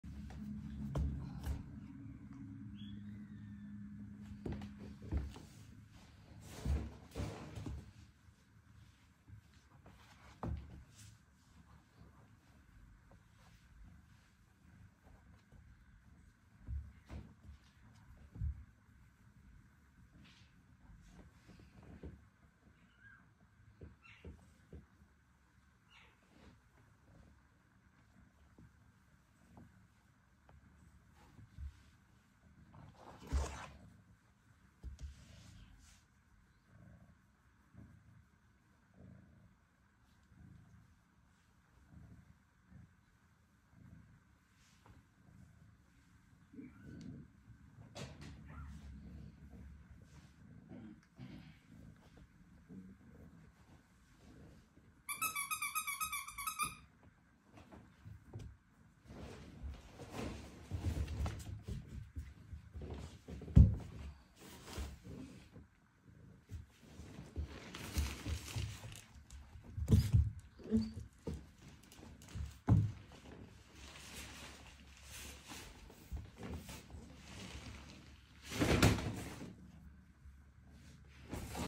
Teaching a puppy to Sit Quietly to be petted: Palisade AKC Standard Poodle
Teaching a puppy to sit quietly to be petted: one of the required skills in our Doggy Kindergarten Program.